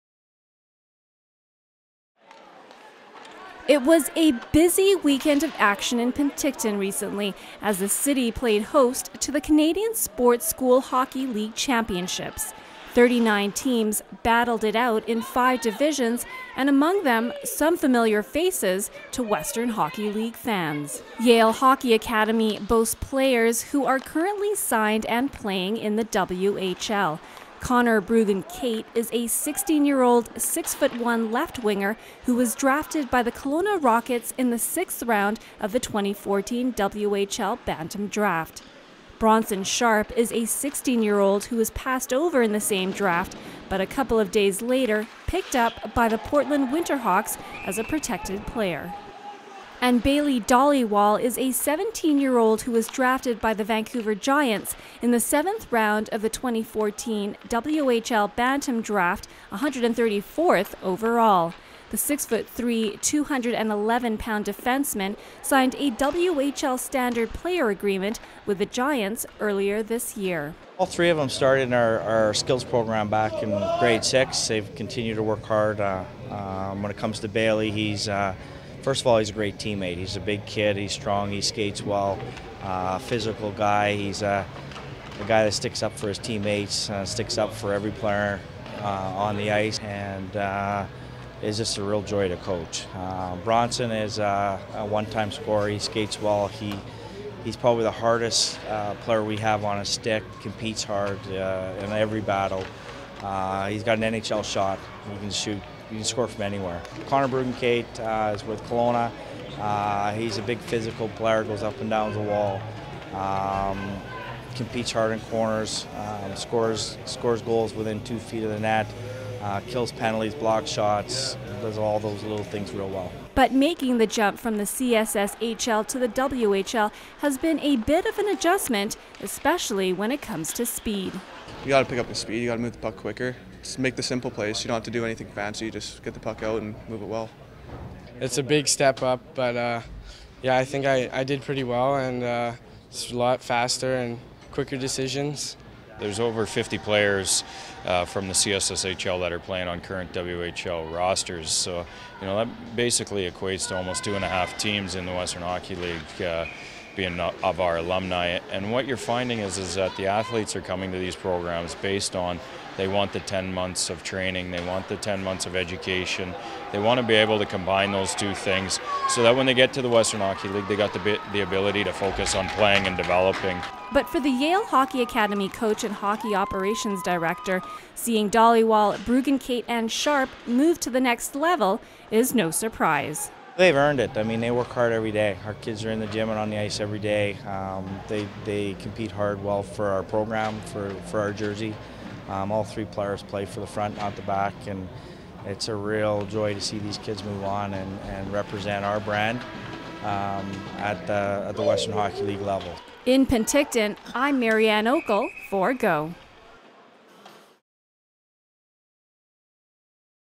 Canadian Sport School Hockey League
The CSSHL Championships were held in Penticton recently and we were there to check out the action.